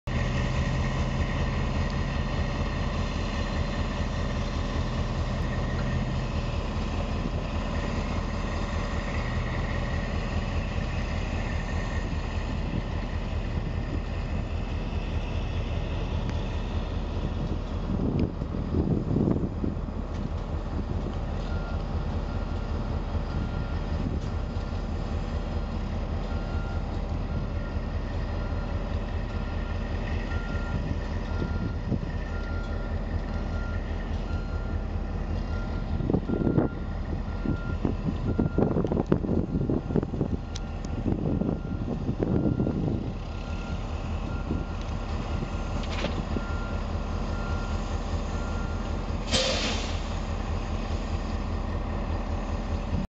CPR MOW Low Bed Transport Truck
Low bed truck getting ready to load up a tamper in the Castlegar wye.